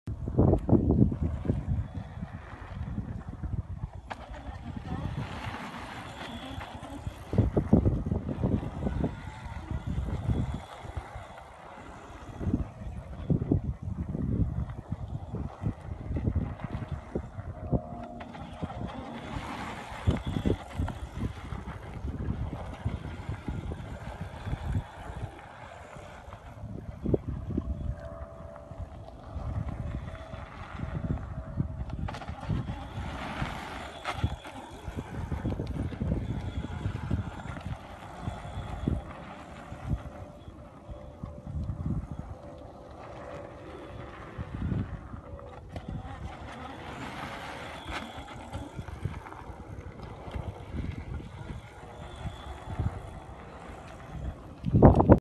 Fat Tire Ebike Offroad
Homebuilt fat tire ebike on test drive offroad. Ebike is capable of 26
mph and 22 miles per charge. Batteries are 50 volt 16 Ah lipos